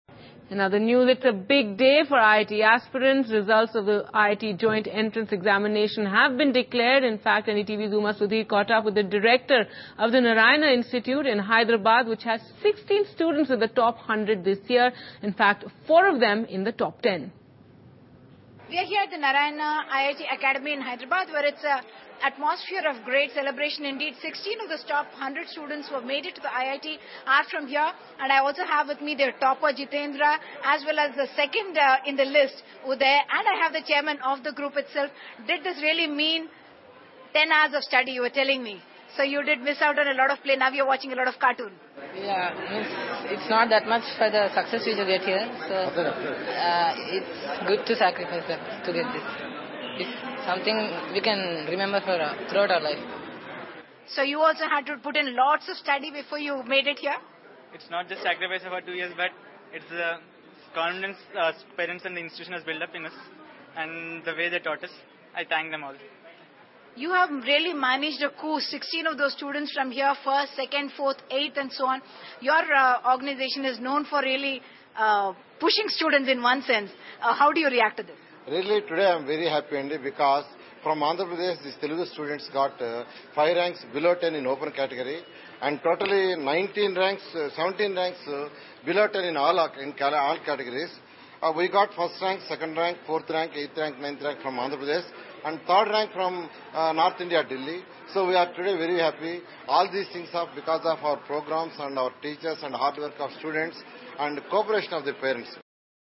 Meet the IIT JEE toppers.flv
Uday Kantilal Shah comes 2nd in IIT JEE 2010 and is interviewed by NDTV.